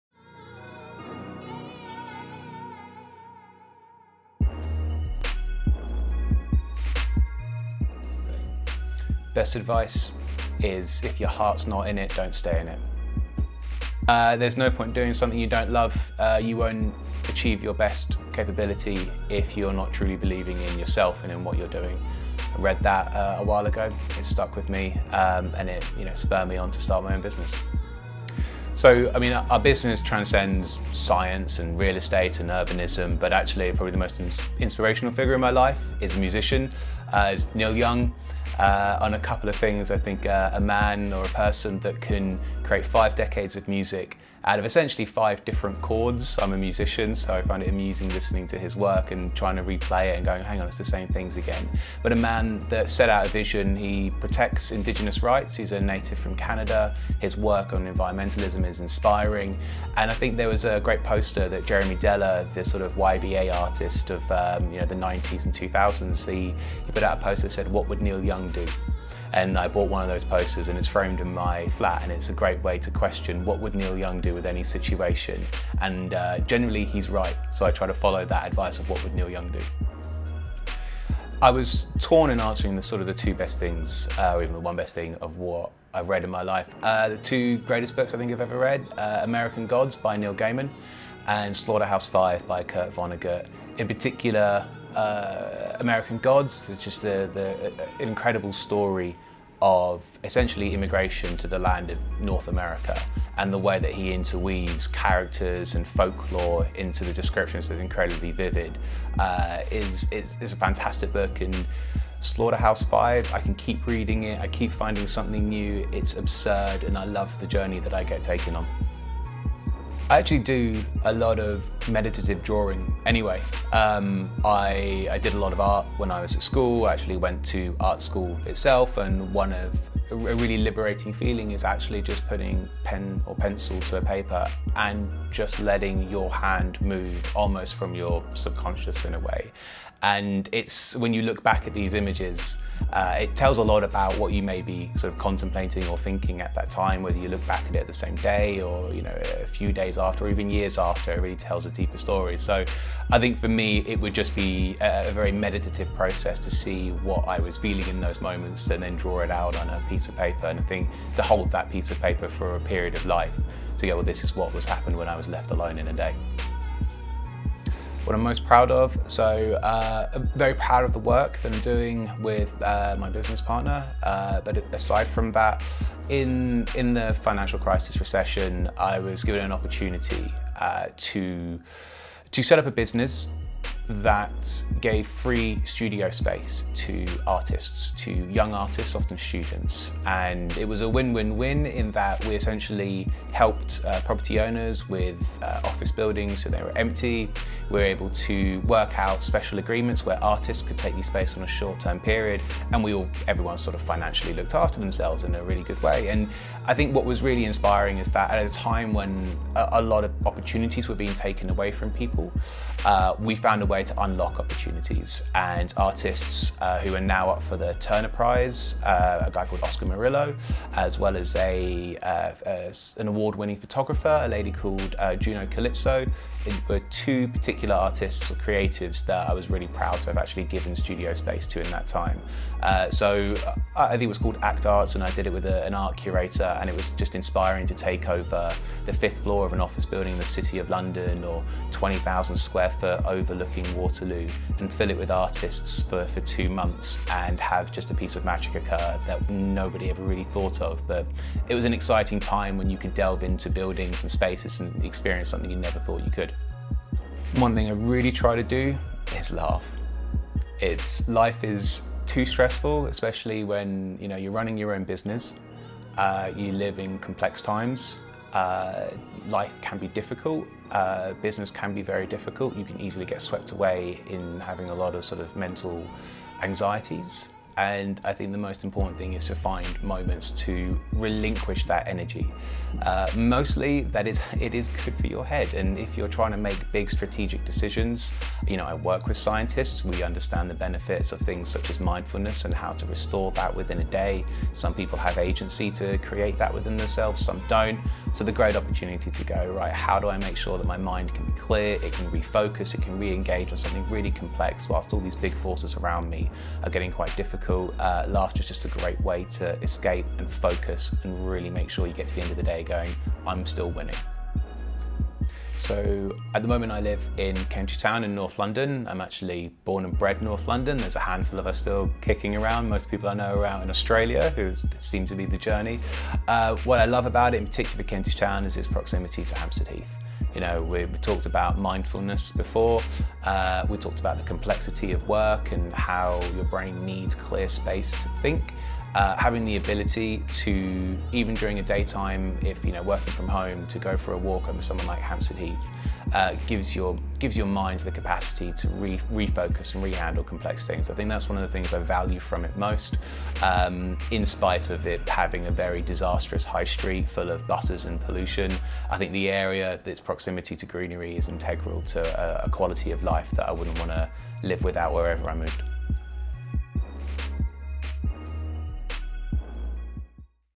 Have you met Josh? | Centric Lab Co-Founder | URBANIST PLATFORM
We sat down for an up close interview with Co-Founder of the Centric Lab, Josh Artus.

Josh has worked at the intersection of real estate, technology and next generation users of space for 10 years. Centric Lab are a company that use neuroscience and urban data to help organisations create healthy and resilient environments.

In this interview, he shares some insight on what inspirations have helped him get to where he is today.

Video produced by CJ Obi and directed by Marcus Hessenberg.